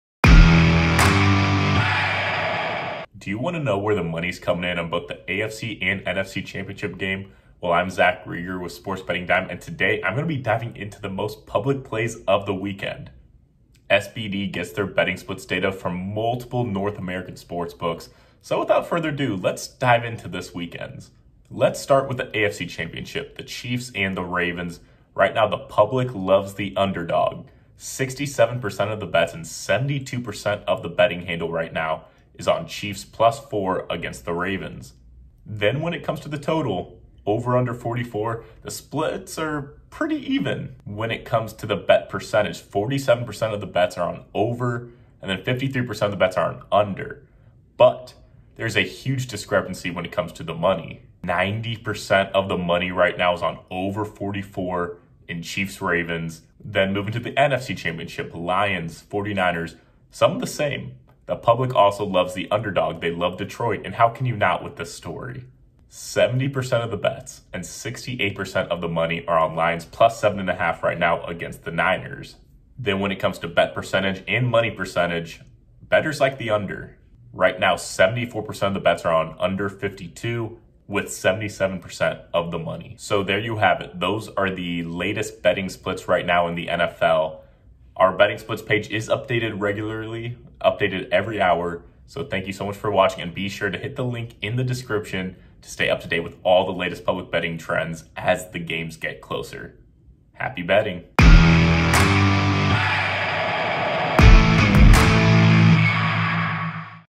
NFL Public Betting & Money Percentages for the Conference Championships
SBD’s Zach Reger dives into the most public bets of the weekend in the AFC and NFC Championship games.

Our NFL betting splits data is updated hourly, so see who the public is betting on here: